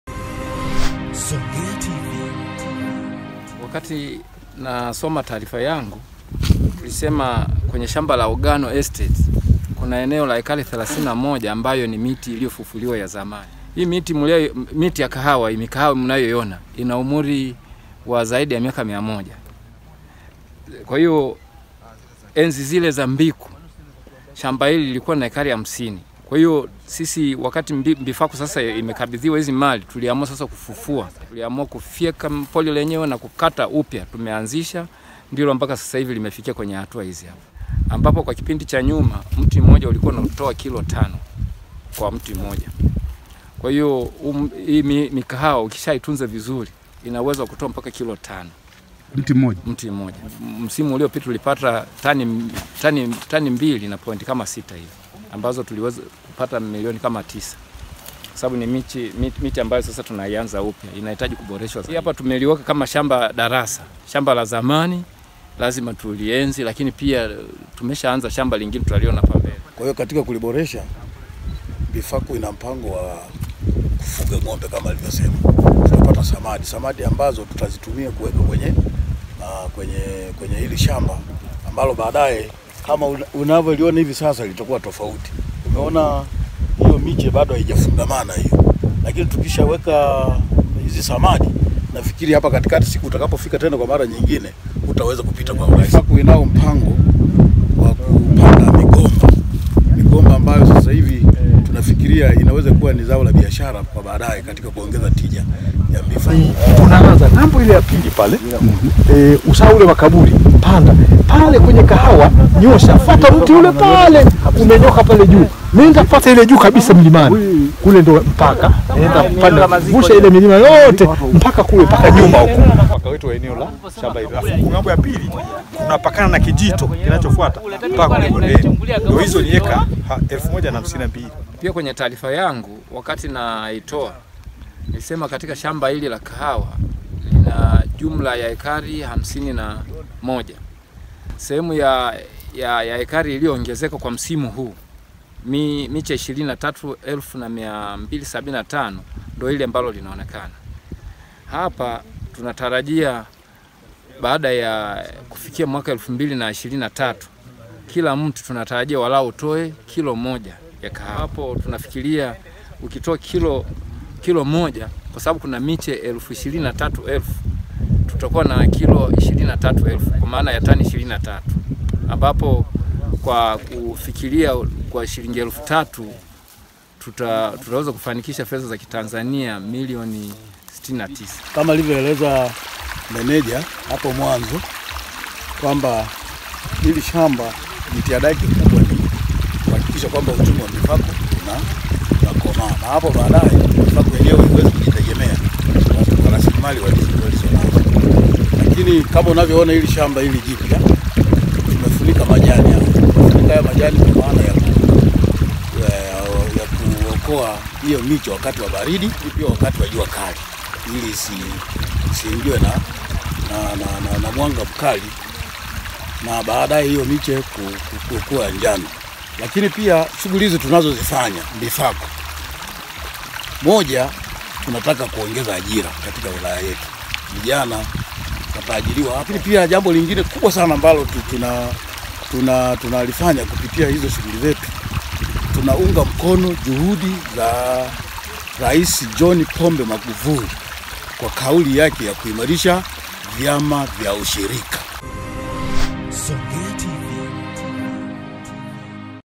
MICHE YA KISASA YA KAHAWA ITAKAVYOONGEZA MAPATO NA KUIMARISHA USHIRIKA MBIFACU
Chama kikuu cha Ushirika katika wilaya za Mbinga na Nyasa MBIFACU kimepanda miche ya kisasa ya kahawa zaidi ya 23,000 ambayo inatarajia kuwaingizia shilingi milioni 69 hivyo kuongeza mapato na kuimarisha USHIRIKA.